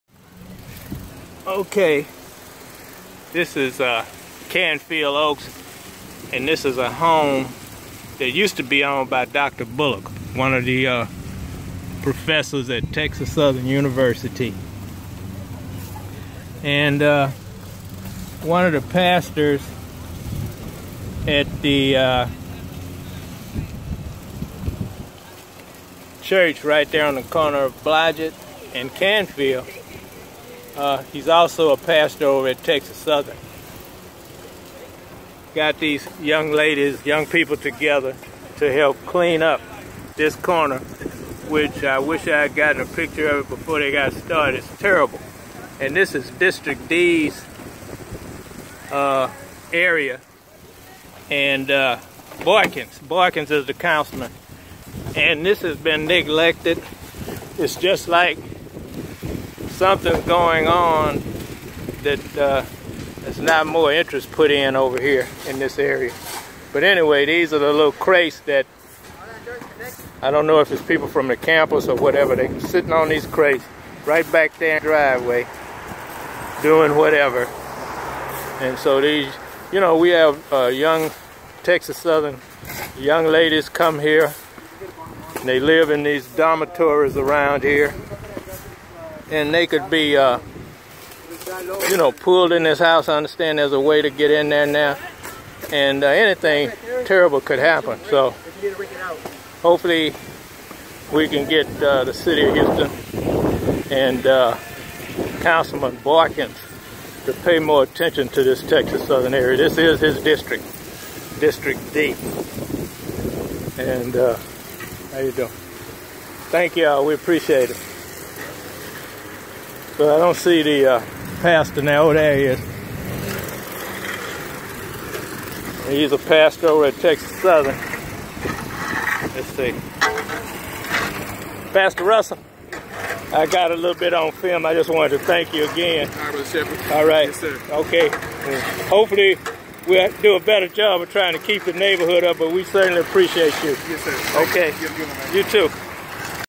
Canfield Oaks District-D, neglected,abandoned home
Danger to students  in Texas Southern University area.